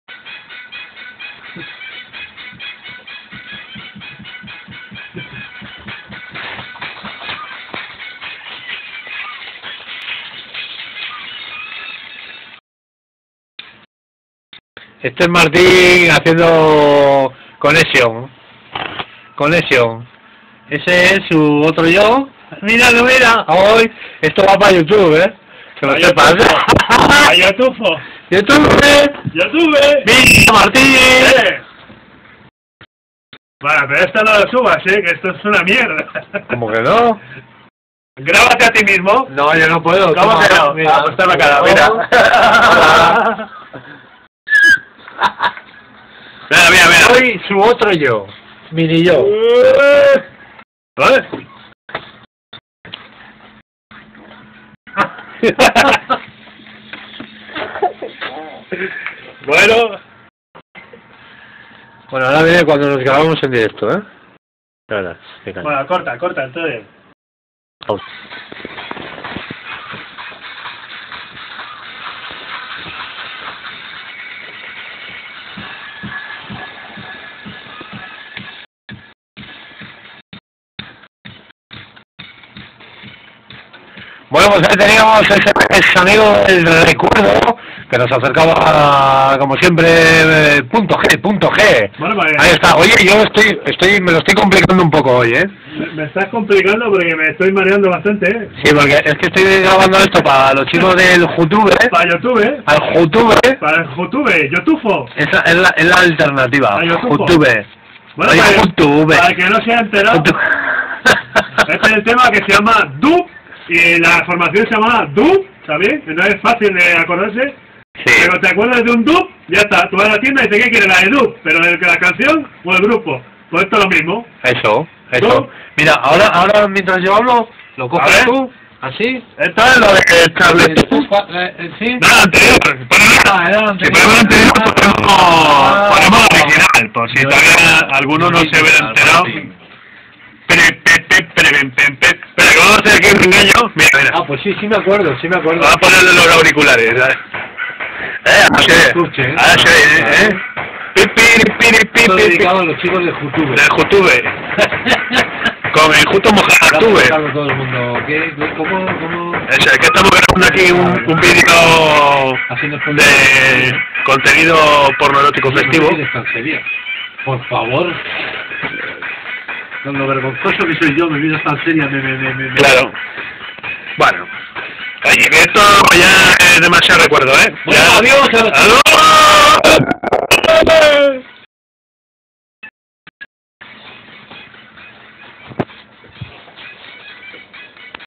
GRABANDO OSCAR GROOVE CONEXION
LA TONTERIA DE GRABARNOS AL TIEMPO QUE REALIZAMOS EL PROGRAMA .